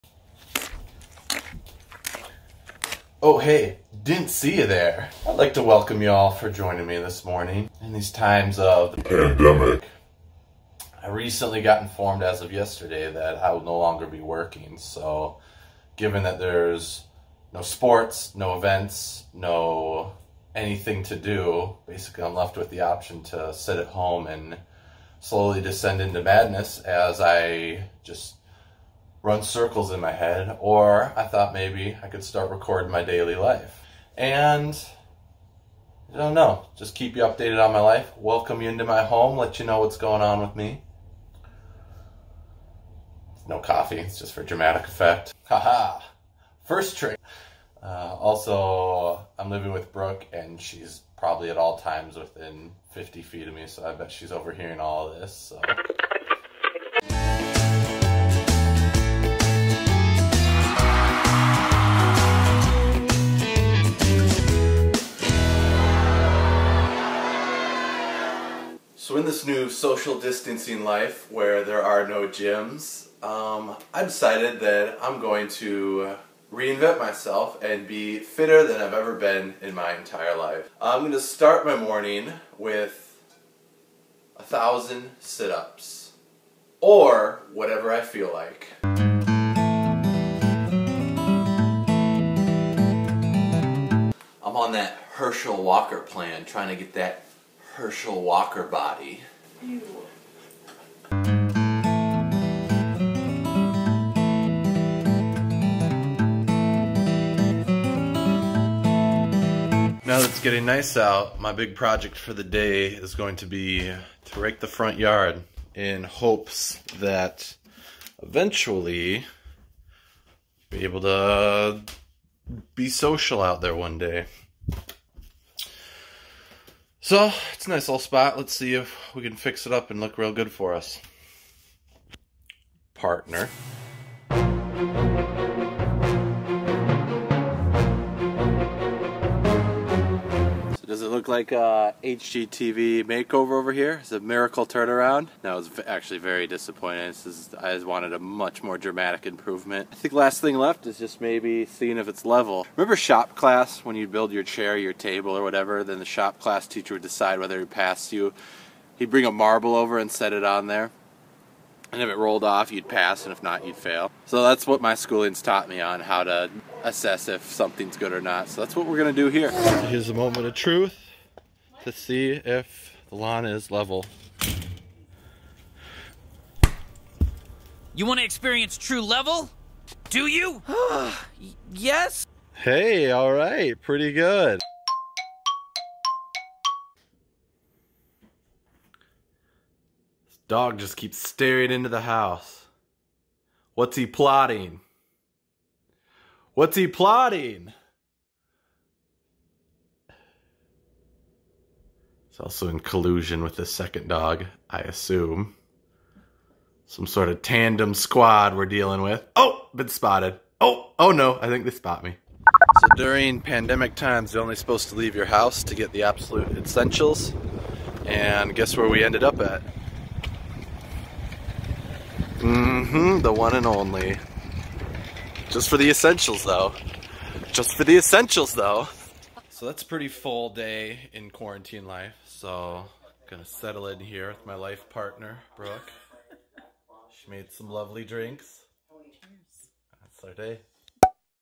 Quarantine Cal Episode 1 - Lawn Care and The Plotting Dogs Next Door
1st day into being placed under stay-at-home order.